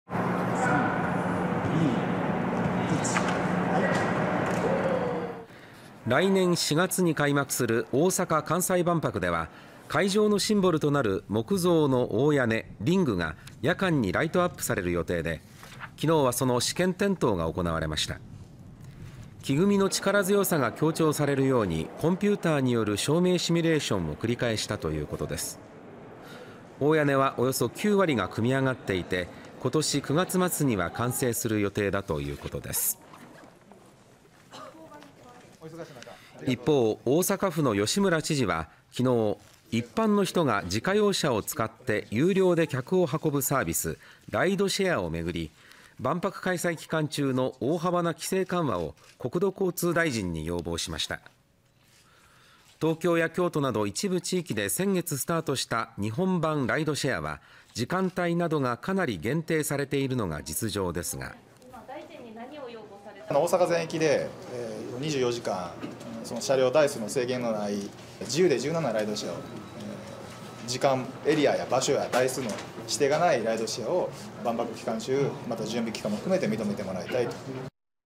『２４時間・台数制限なし』ライドシェア 万博に向け大阪・吉村知事が規制緩和を要望「時間・エリア・台数指定なしを認めて」（2024年5月22日）
大阪府の吉村知事は２１日、一般の人が自家用車を使って有料で客を運ぶサービス「ライドシェア」をめぐり、万博開催期間中の大幅な規制緩和を国土交通大臣に要望しました。東京や京都など一部地域で４月から始まった「日本版ライドシェア」は、時間帯などがかなり限定されているのが実状です。

　（大阪府・吉村洋文知事）「大阪全域で２４時間、車両台数の制限のない、自由で柔軟なライドシェア、時間・エリア・台数の指定のないライドシェアを万博期間中・準備期間も含めて認めてもらいたい」

　一方、来年の大阪・関西万博で会場のシンボルとなる「大屋根リング」の夜間ライトアップが公開されました。

　来年４月に開幕する大阪・関西万博では会場のシンボルとなる木造の「大屋根（リング）」が夜間にライトアップされる予定で、２１日はその試験点灯が行われました。

　木組みの力強さが強調されるように、コンピューターによる照明シミュレーションを繰り返したということです。

　大屋根は約９割が組み上がっていて、今年９月末には完成する予定だということです。